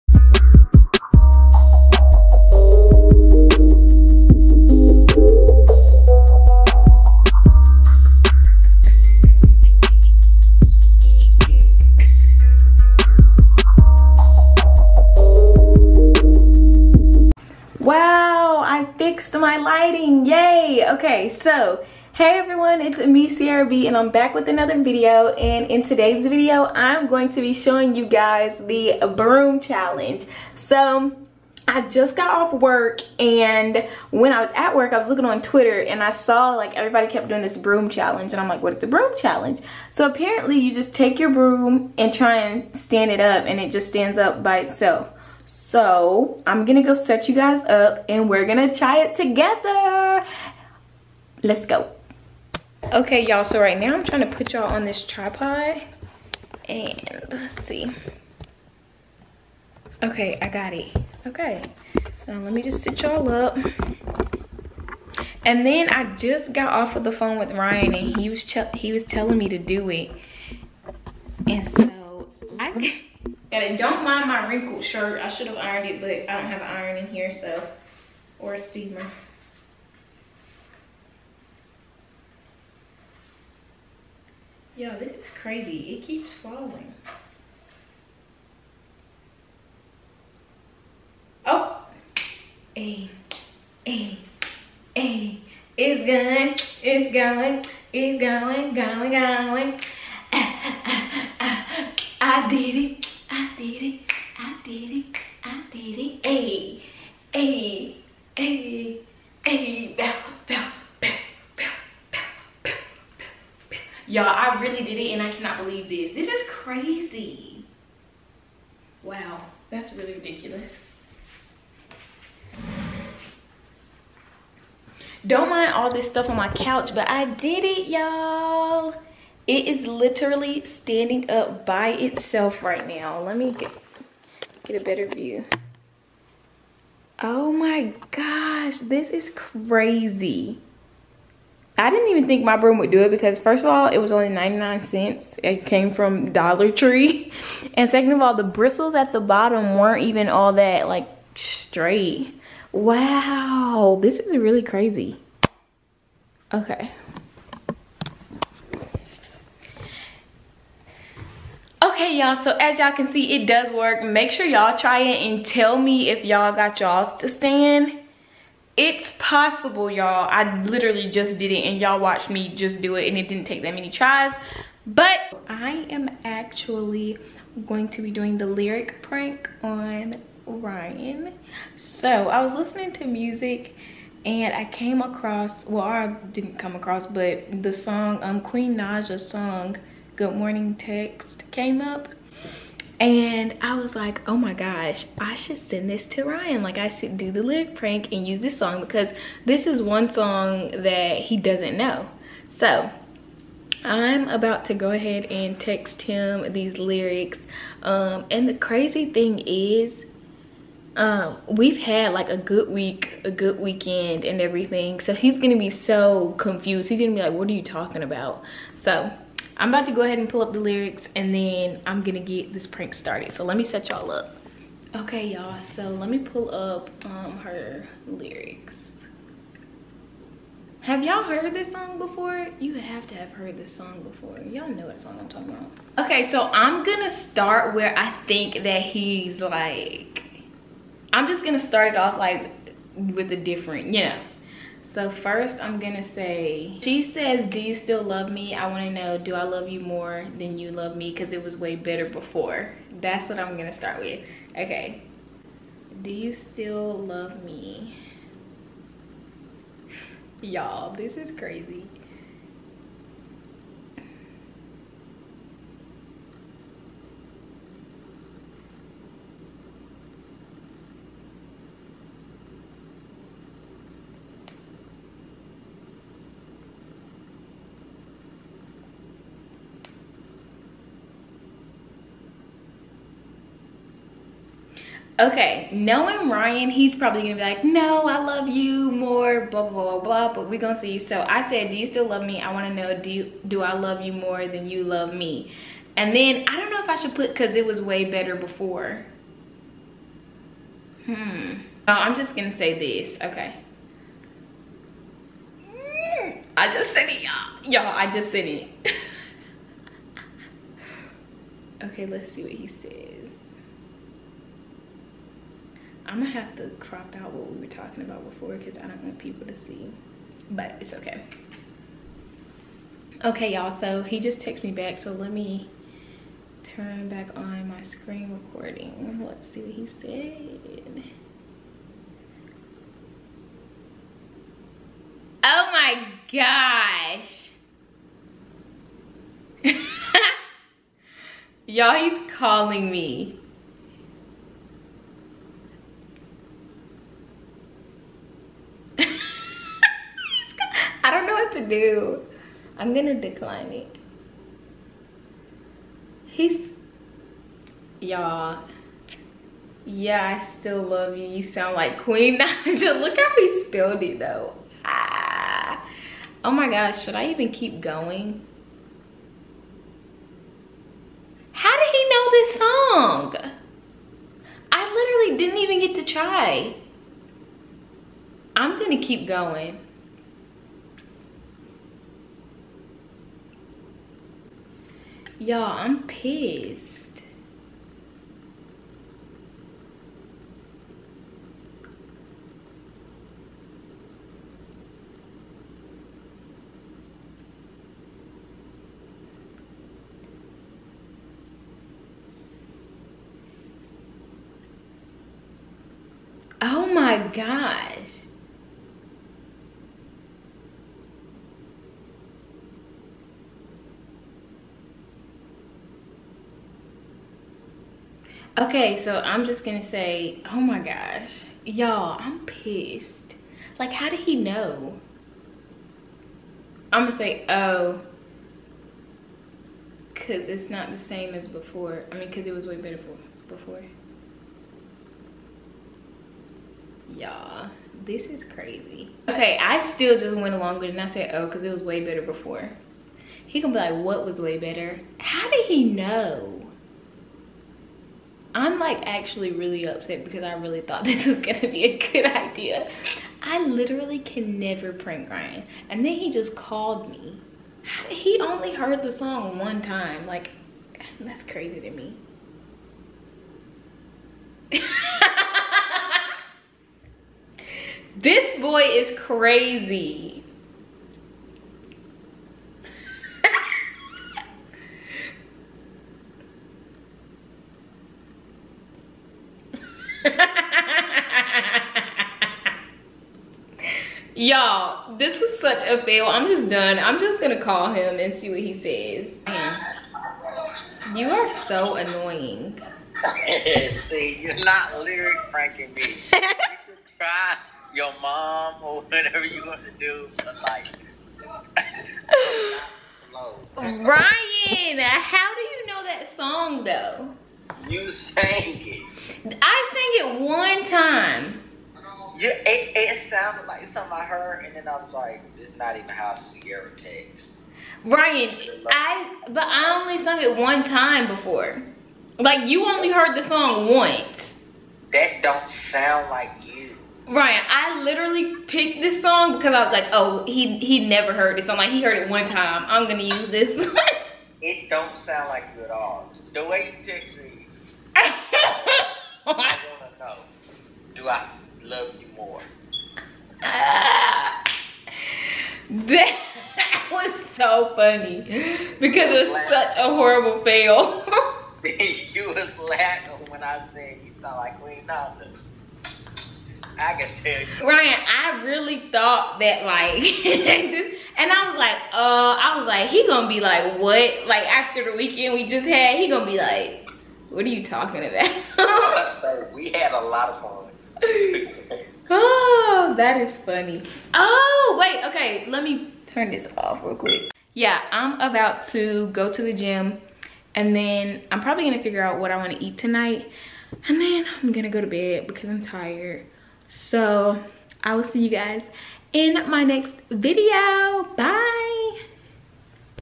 LYRIC PRANK ON BOYFRIEND| EPIC FAIL | Ciera B.
Hey guys! I did a lyric prank on boyfriend!! MUST WATCH!!! It is hilarious and went horribly WRONG.

FAQ:
Q: How old am I?
A: 23

Q: Where am I from?
A: NC